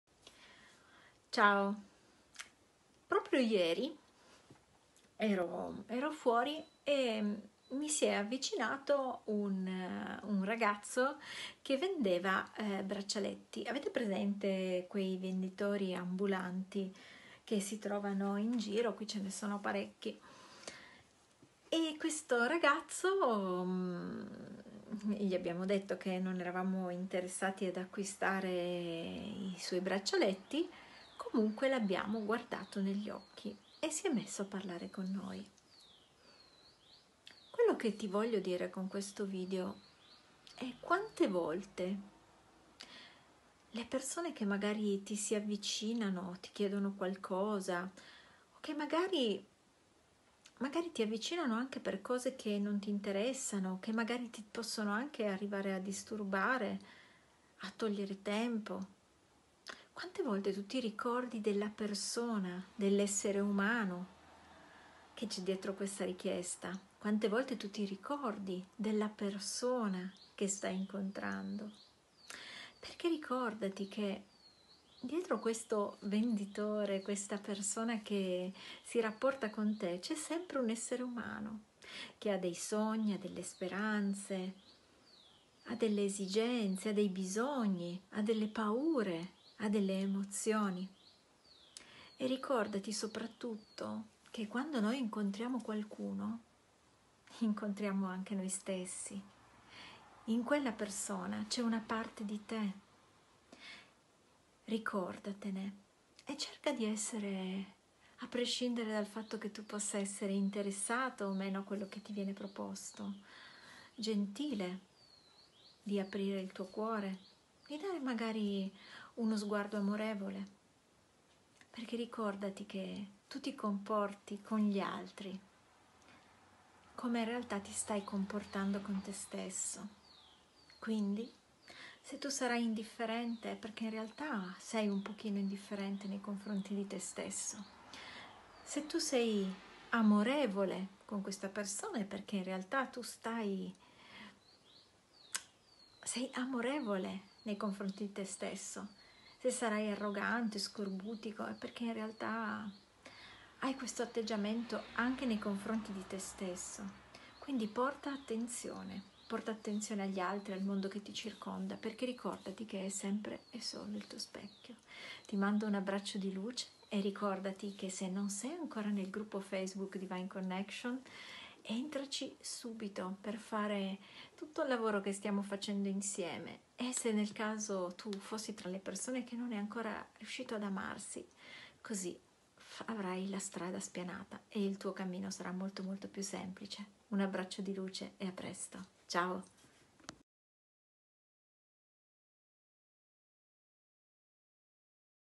23ispirazione APRI IL TUO CUORE QUANDO INCONTRI IL PROSSIMO
Quante volte tu ti ricordi della persona che sta dietro il venditore che stai incontrando?

Il comportamento che hai verso gli altri riflette l’atteggiamento che riservi anche a te stesso/a…